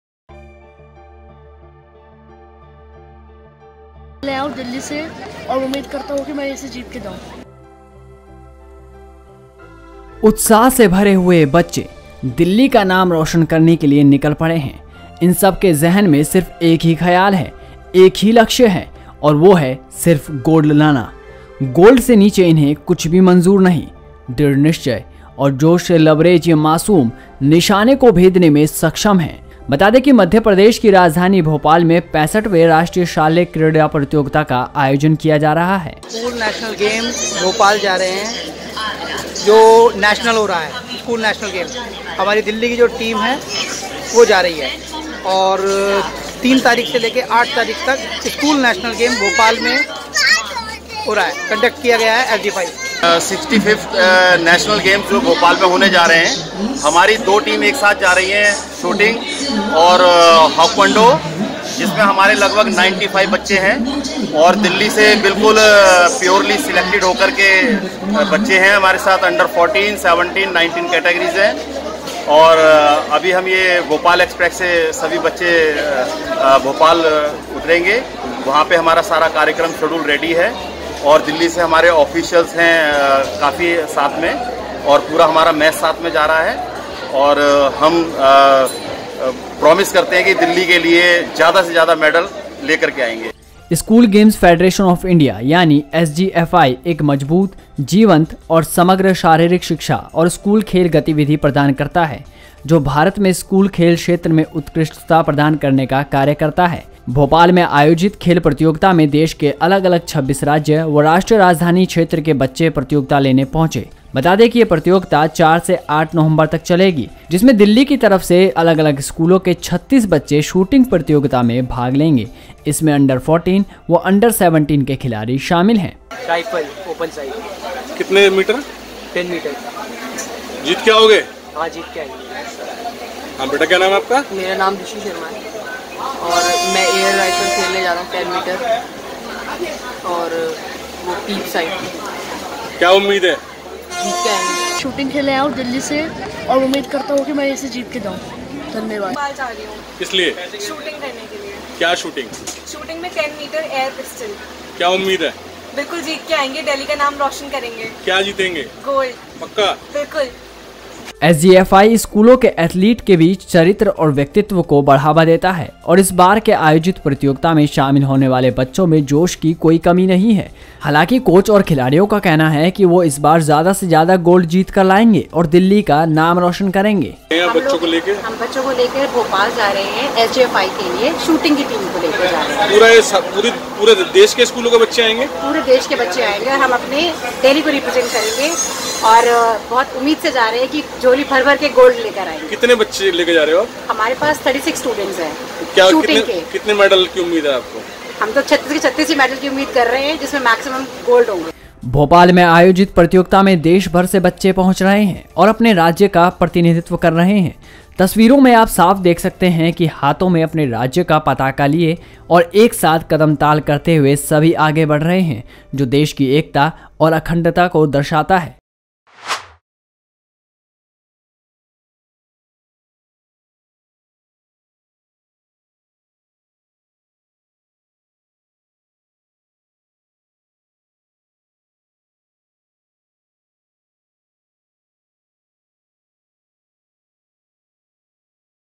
भोपाल नेशनल शूटिंग चैंपियनशिप में भाग लेने दिल्ली के बच्चे हुए रवाना | Punjab Kesari
भोपाल नेशनल शूटिंग चैंपियनशिप में भाग लेने दिल्ली के बच्चे हुए रवाना |